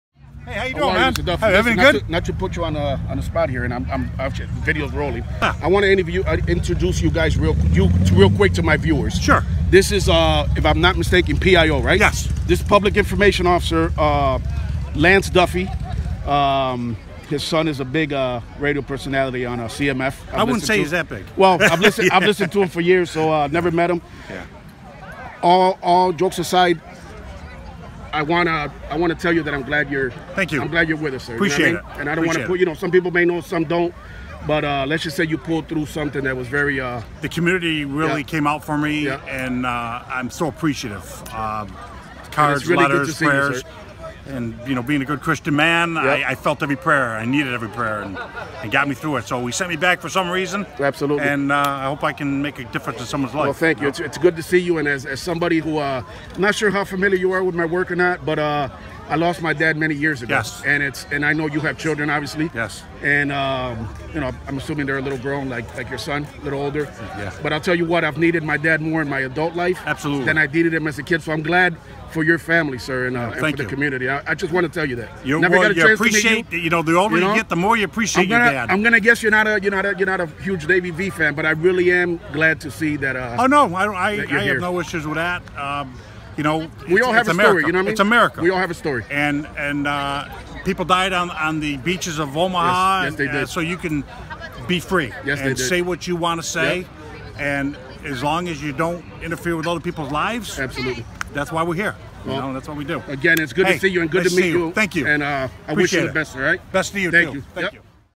Gates, NY Police Public Information Officer Lance Duffy Talks with DavyVTV
Davy V. speaks with Gates, NY Public Information Officer Lance Duffy

Follow Davy V. on twitter

"Like" and Follow Davy V. on Facebook